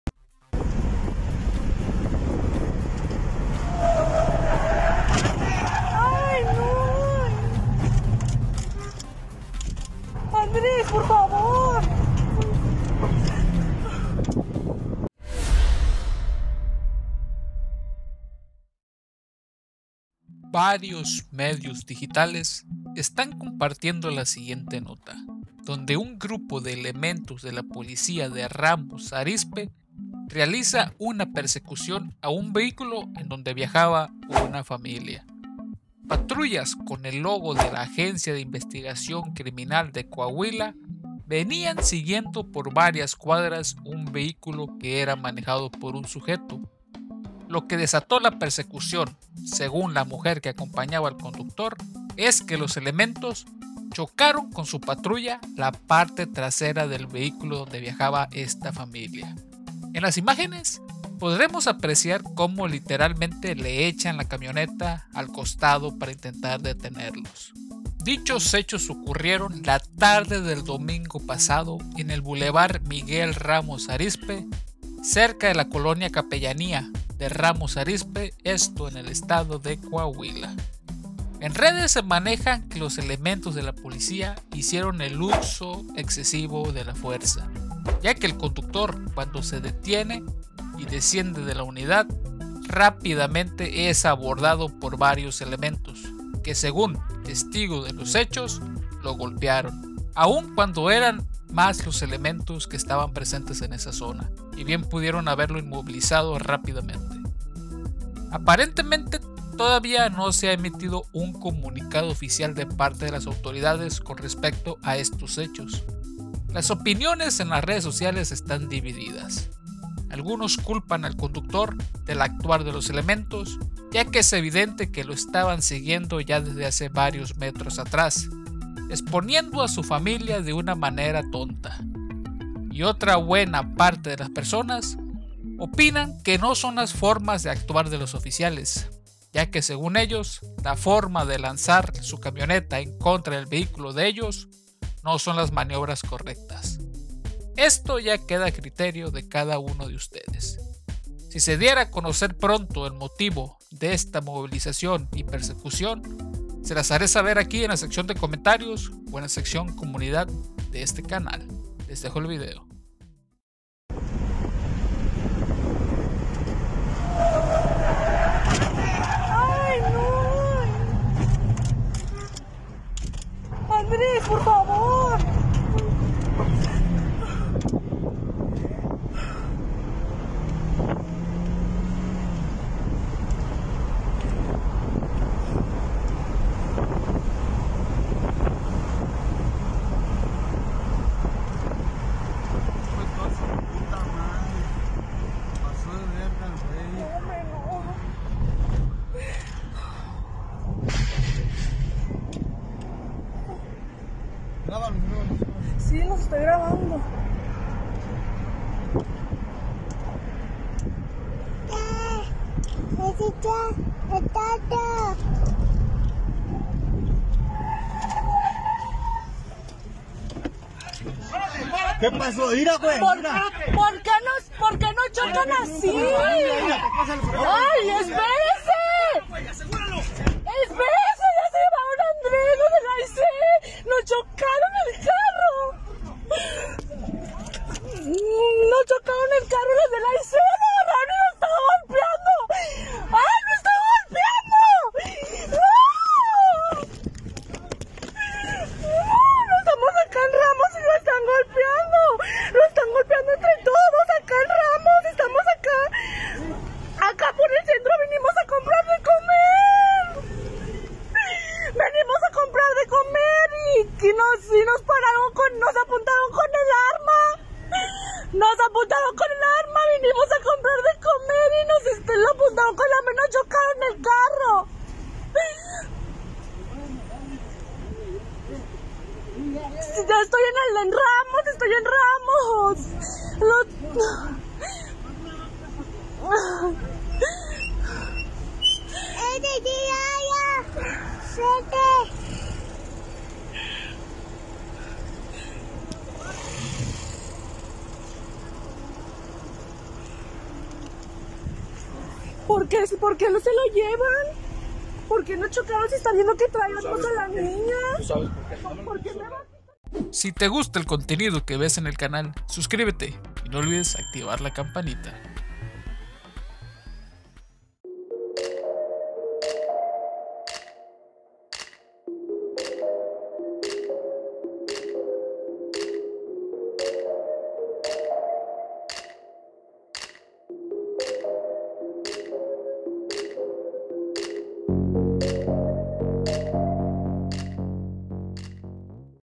¿Elementos harían uso desmesurado de la Fuerza en Ramos Arizpe?, Tu que Opinas
Coahuila.- Unidades con el logo de la Agencia de Investigación de Coahuila, venían siguiendo por varias cuadras un vehículo que era manejado por un sujeto, lo que desato la persecución según la mujer que acompañaba al conductor, es que los elementos chocaron la parte trasera de su vehículo, en las imágenes podremos apreciar como literalmente le echan la camioneta a un costado para intentar detenerlo.

Dichos hechos ocurrieron la tarde del domingo pasado en el Boulevard Miguel Ramos Arizpe, cerca de la colonia Capellanía, de Ramos Arizpe en el estado de Coahuila.

¿ Tu que opinas?

Capítulos:
0:00 Resumen
0:15 Disclaimer
0:20 Inicio de la Nota
2:43 Video Integro
5:17 Pantalla Final

* Alguna información dentro del video íntegro puede no mostrarse por ser de carácter personal.

- El canal se creo con la intención de informar a toda la población del territorio mexicano desde el año 2013.

📚 Libros recomendados:

🎶 Licencia musical: